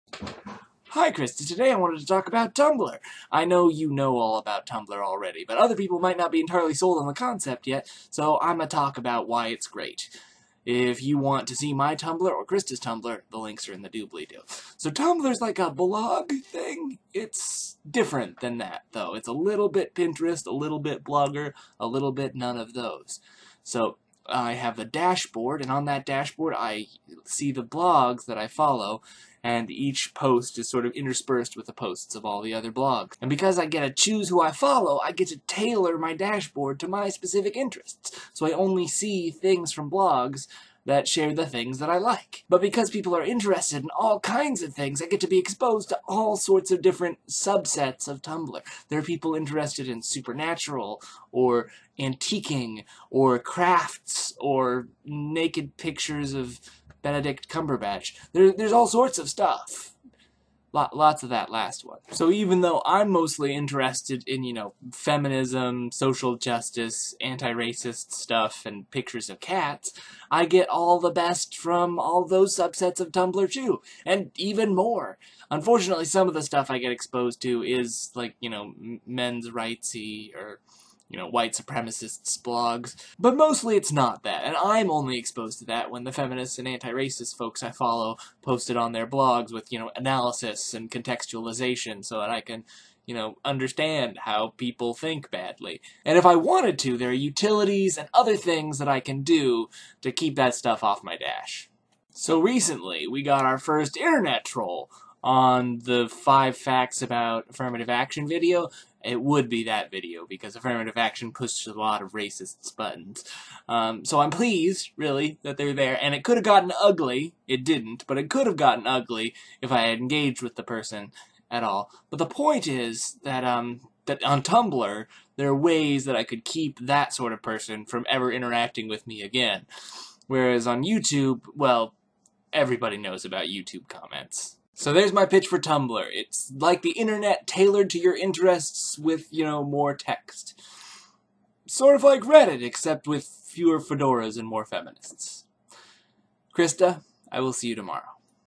Tumblr
In which Samantha extolls the virtues of Tumblr.